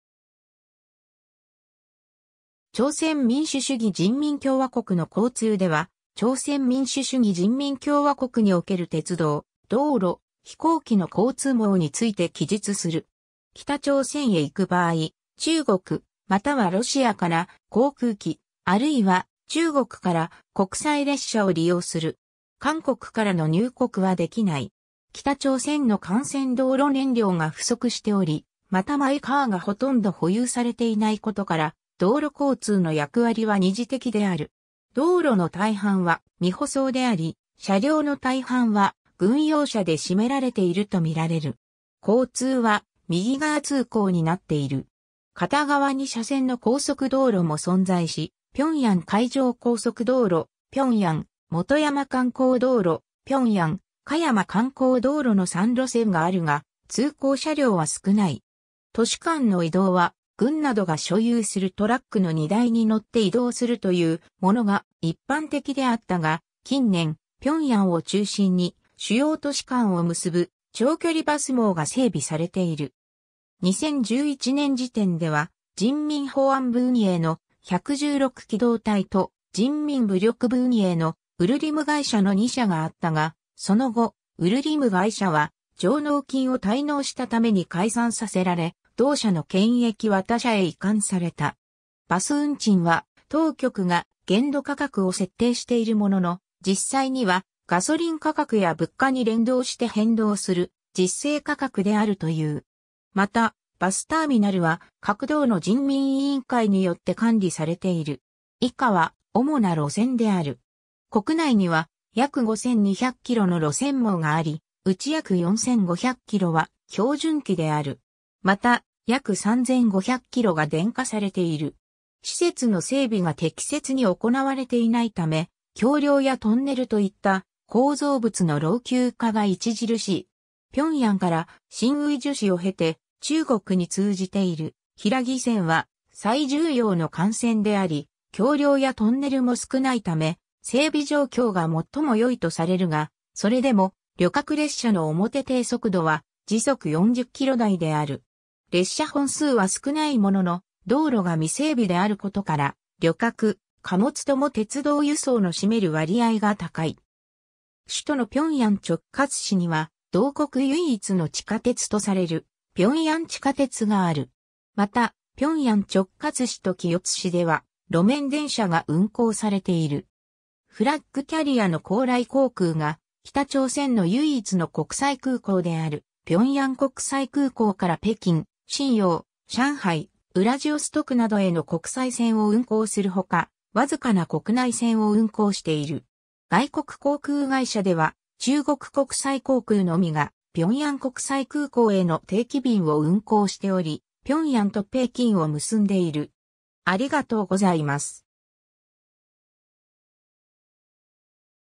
朝鮮民主主義人民共和国の交通
朝鮮民主主義人民共和国の交通（ちょうせんみんしゅしゅぎじんみんきょうわこくのこうつう）では、朝鮮民主主義人民共和国（北朝鮮）における鉄道、道路、飛行機の交通網について記述する。
 北朝鮮へ行く場合、中国またはロシアから航空機、あるいは中国から国際列車を利用する。
韓国からの入国（入境）はできない。
 北朝鮮の幹線道路 燃料が不足しており、またマイカーがほとんど保有されていないことから、道路交通の役割は二次的である。
道路の大半は未舗装であり、車両の大半は軍用車で占められているとみられる。
交通は右側通行になっている。
片側2車線の高速道路も存在し、平壌-開城高速道路、平壌-元山観光道路、平壌-香山観光道路の3路線があるが、通行車両は少ない。
 都市間の移動は、軍などが所有するトラックの荷台に乗って移動するというものが一般的であったが、近年、平壌を中心に、主要都市間を結ぶ長距離バス網が整備されている。
2011年時点では、人民保安部（警察）運営の「116機動隊」と、人民武力部（軍）運営の「ウルリム会社」の2社があったが、その後、「ウルリム会社」は上納金を滞納したために解散させられ、同社の権益は他社へ移管された。
バス運賃は、当局が「限度価格」を設定しているものの、実際にはガソリン価格や物価に連動して変動する「実勢価格」であるという。
また、バスターミナルは各道の人民委員会によって管理されている。
 以下は、主な路線である。
 国内には約5,200キロの路線網があり、うち約4,500キロは標準軌である。
また約3,500キロが電化されている。
 施設の整備が適切に行われていないため、橋梁やトンネルといった構造物の老朽化が著しい。
平壌から新義州市を経て中国に通じている平義線は最重要の幹線であり、橋梁やトンネルも少ないため、整備状況がもっともよいとされるが、それでも旅客列車の表定速度は時速40キロ台である。
 列車本数は少ないものの、道路が未整備であることから、旅客、貨物とも鉄道輸送の占める割合が高い。
 首都の平壌直轄市には、同国唯一の地下鉄とされる平壌地下鉄がある。
また、平壌直轄市と清津市では、路面電車が運行されている。
 フラッグ・キャリアの高麗航空が、北朝鮮の唯一の国際空港である平壌国際空港（順安空港）から北京、瀋陽、上海、ウラジオストクなどへの国際線を運航するほか、わずかな国内線を運航している。
外国航空会社では中国国際航空のみが平壌国際空港への定期便を運航しており、平壌と北京を結んでいる。